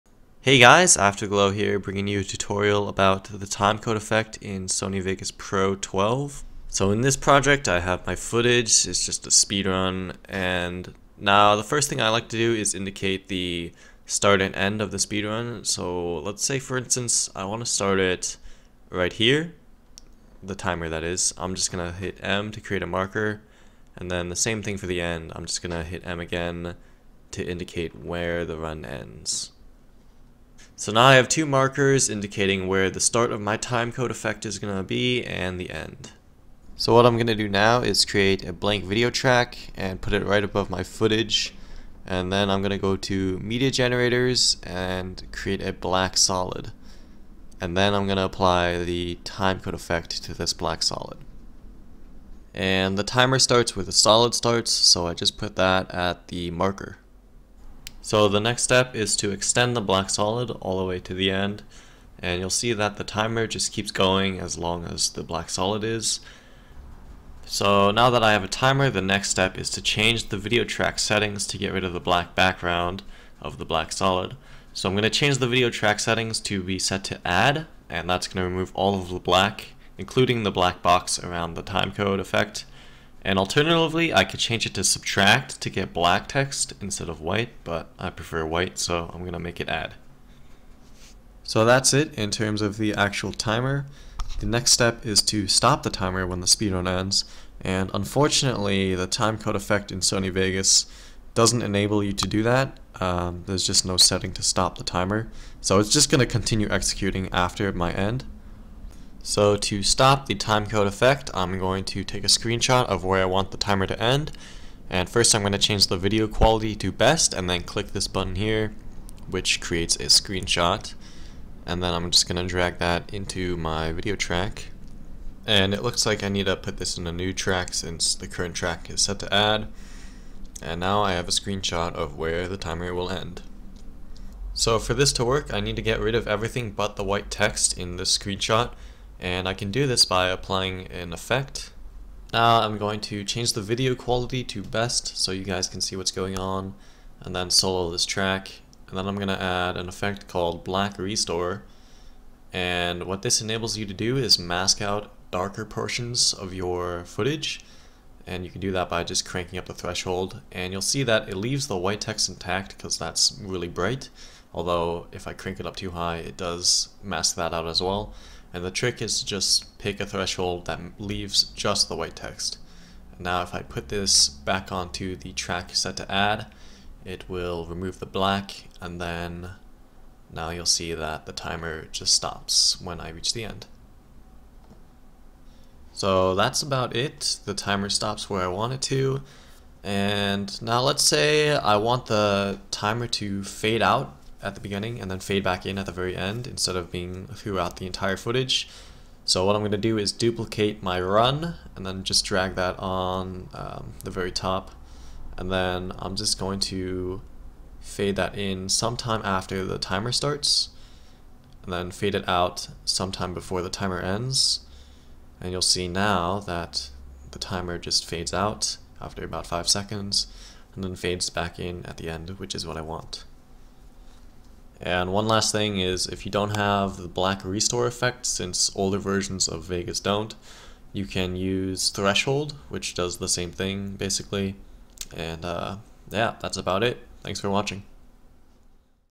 Sony Vegas Pro 12 Tutorial - Using Timecode to create a timer that stops
I found a good way to create a timer that stops using the Timecode effect in Sony Vegas so Ihere's a tutorial about it. I'll try to answer any questions. Let me know if there's any topics that you guys want a tutorial on.